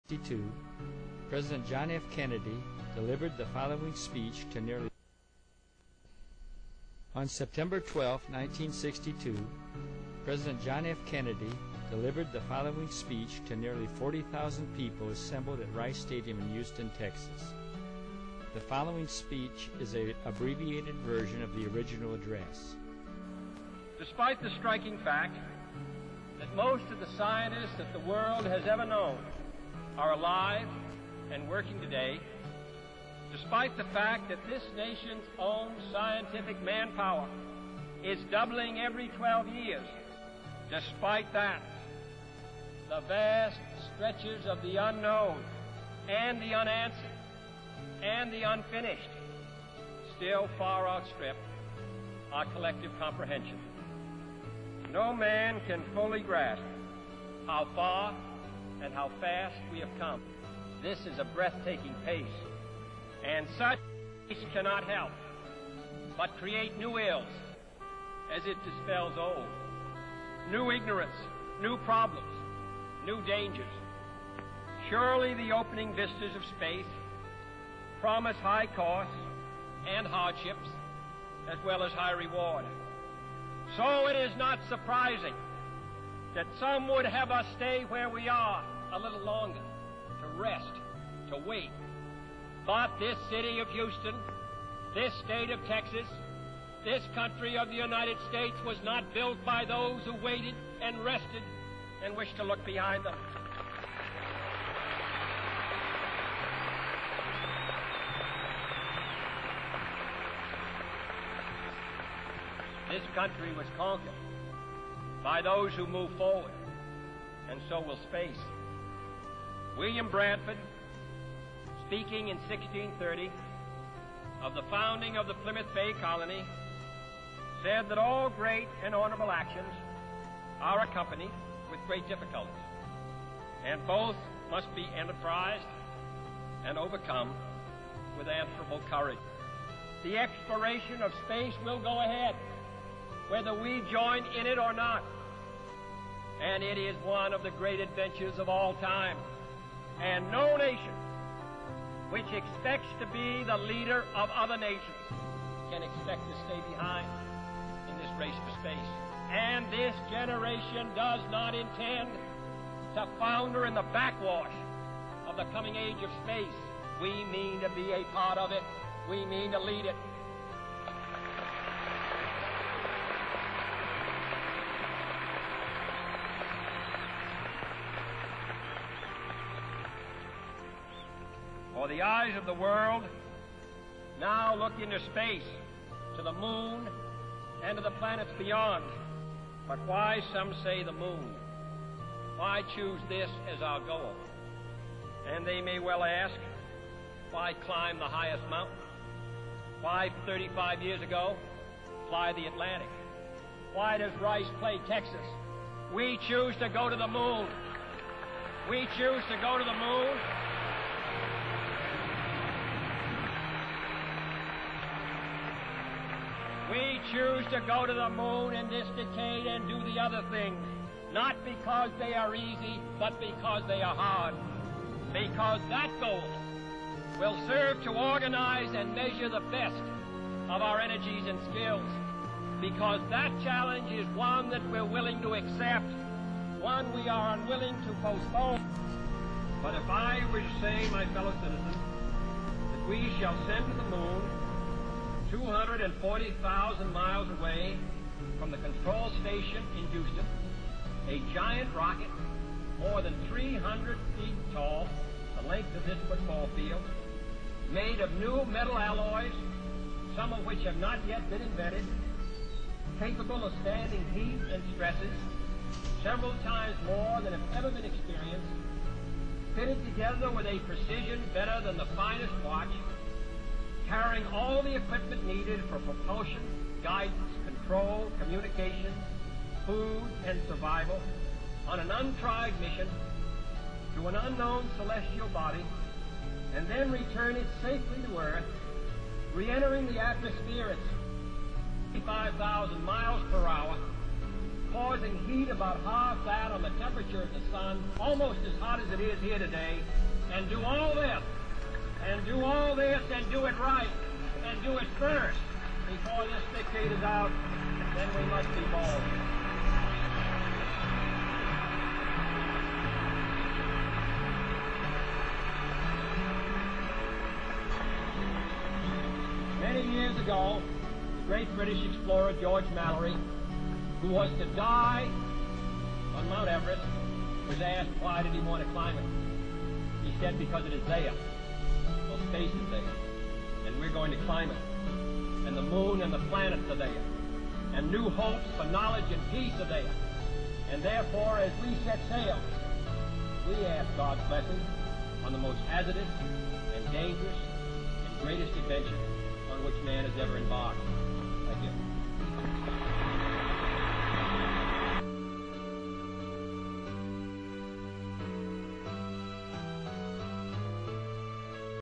JFK's 'Go to the Moon In This Decade' Speech at Rice University
Watch this excerpt of President Kennedy's famous 1962 speech at Rice University where he sets forth the bold and exciting goal of going "to the Moon in this decade."

Indeed, in just ten years, the United States went from no human space program through three generations of manned spaceships (Mercury, Gemini and Apollo) to land safely on the Moon and return.  We did the impossible then, now let's use our 50-year -newer technology to go to Mars, back to the Moon, and on to asteroids, comets and beyond!